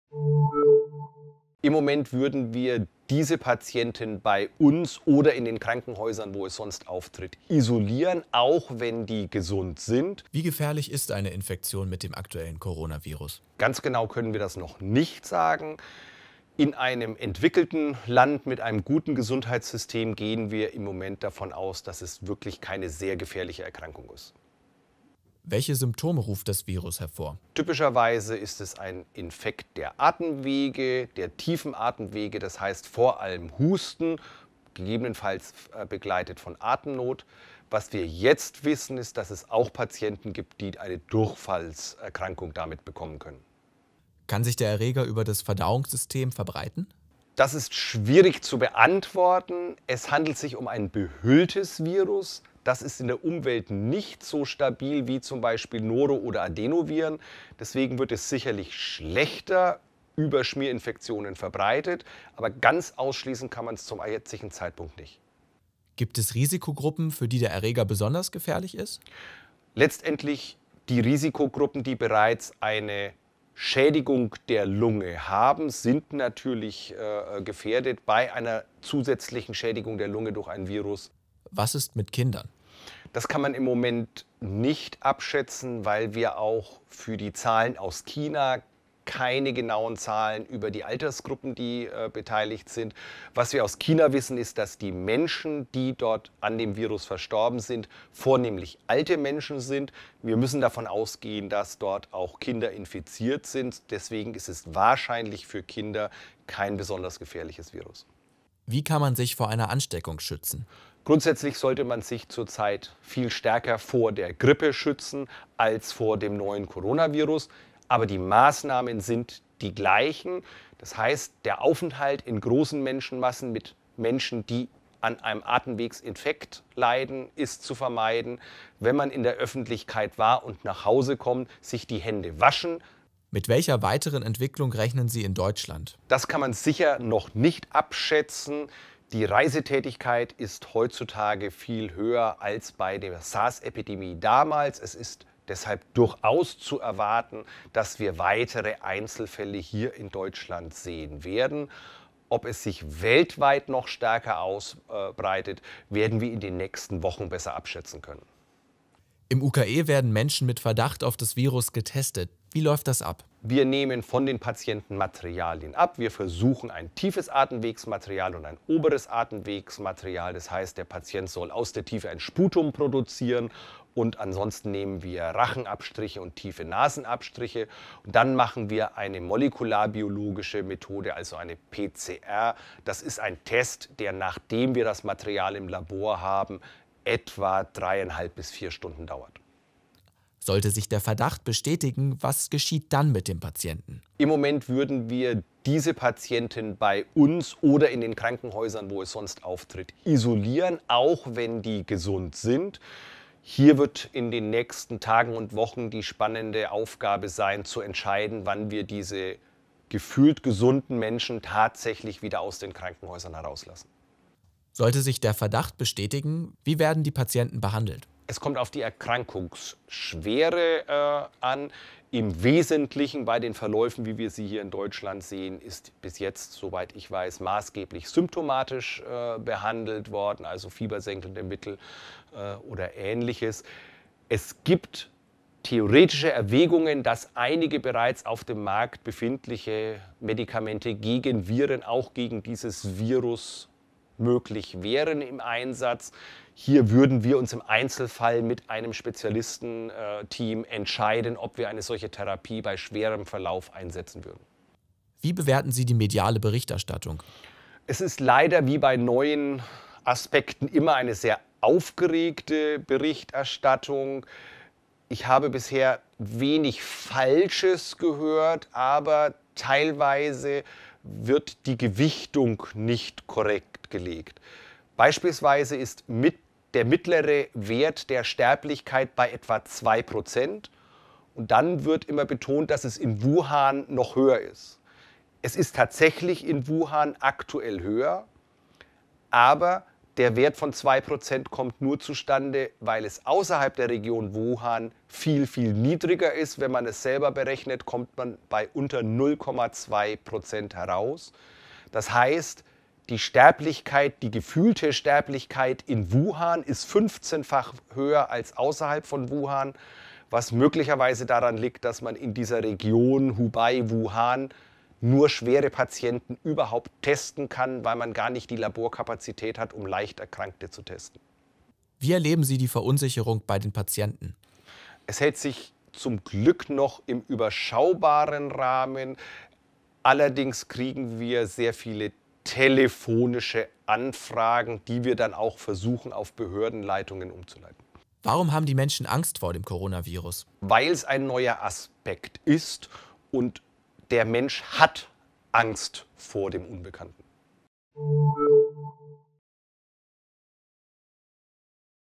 Symptome, Schutz, Übertragung: Facharzt beantwortet die wichtigsten Fragen zum Coronavirus
Wie gefährlich ist das Coronavirus? Wie kann man sich schützen? Prof. Dr. med. Johannes Knobloch vom Universitätsklinikum Hamburg-Eppendorf beantwortet im stern-Interview die wichtigsten Fragen.